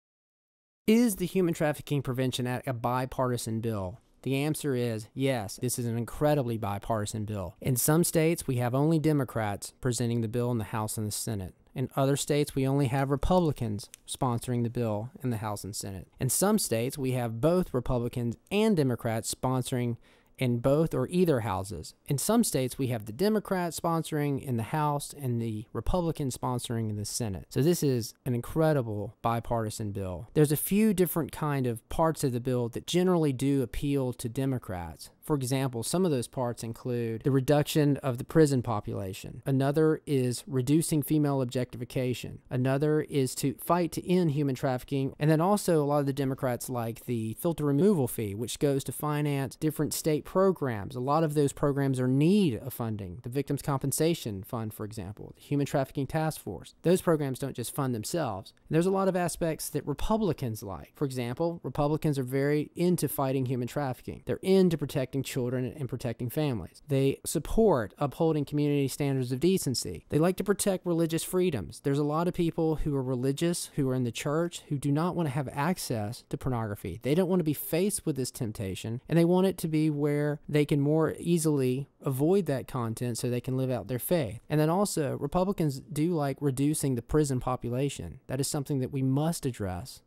Is the Human Trafficking Prevention Act a bipartisan bill?
Humantraffickingpreventionact.com

Question: Is the Human Trafficking Prevention Act a bipartisan bill?
This act is bipartisan. In some states, only Democrats are sponsoring the bill in the house and senate, in other states, only the Republicans are. However, in some states both Democrats and Republicans are sponsoring the bill together. In some states the Democrats are presenting the bill in the house and Republicans in the Senate​. We have yet to find a single politician - Republican or Democrat - who believes that the continued distribution of pornography through products that distribute the internet to minors is a good idea. By this point, basically every family has been adversely impacted by internet pornography in some way - both liberal and conservative.
There are certain aspects of this act that typically appeal to Democrats. For example, Democrats favor (1) fighting human trafficking; (2) reducing female objectification; (3) having a valid government revenue that finances state programs like the victims compensation fund; and (4) reducing the prison population. There are certain aspects of this act that regularly appeal to Republicans. For example, Republicans favor (1) fighting human trafficking; (2) protecting children and families, (3) upholding community standards of decency, (4) protecting the religious freedoms of consumers who do not want to be influenced by pornographic materials, and (5) reducing the prison population.